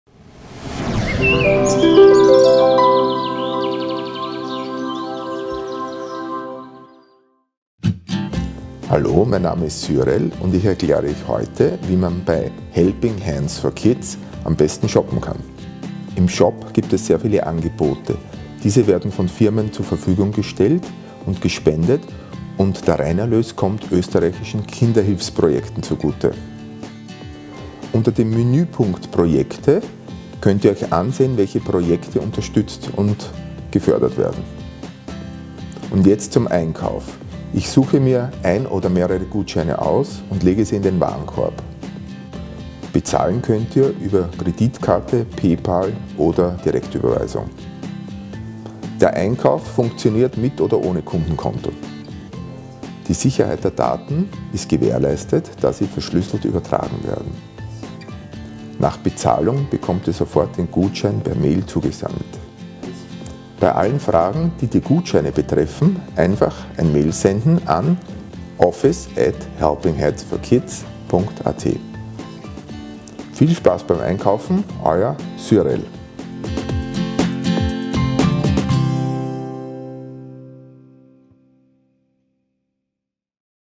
Helping Hands for Kids F.A.Q
Woher kommen die Angebote? Wie kann ich einkaufen? Cyril Radlher beantwortet Eure Fragen.
"Helping Hands for Kids“ ist ein karitativer Spendensammelverein, der durch ein innovatives Finanzierungskonzept zusätzliches Spendengeld für bestehende und neue Kinderhilfsprojekte in Österreich sammelt.